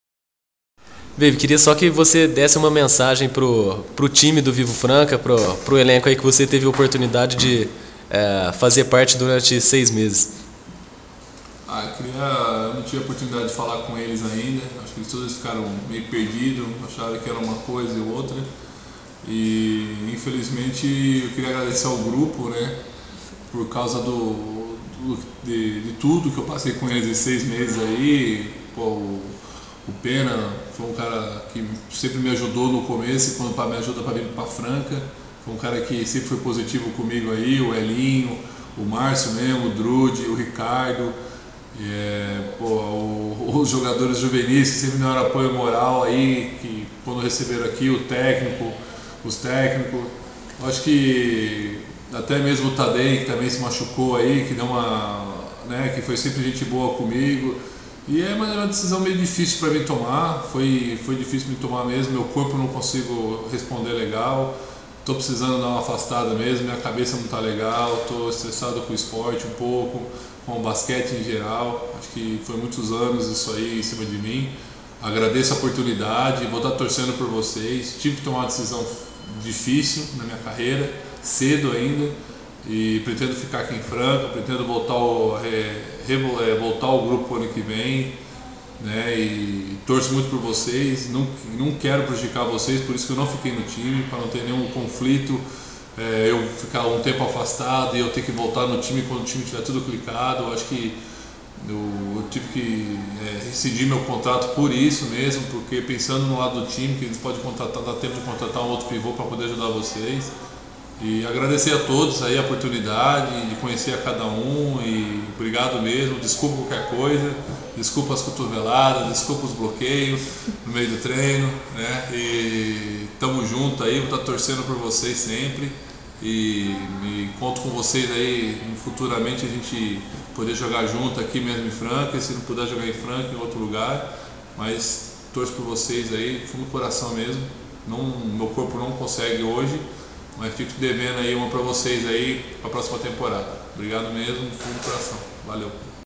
Rafael Bábby manda recado aos jogadores do VIVO/Franca
Após rescindir contrato com equipe francana para recuperar saúde, pivô esclareceu que não possui problemas com outros jogadores do time, comissão técnica ou diretoria. Bábby destacou que torcerá pelo VIVO/Franca durante o NBB e que permanecerá morando em Franca e comparecendo aos jogos no Pedrocão.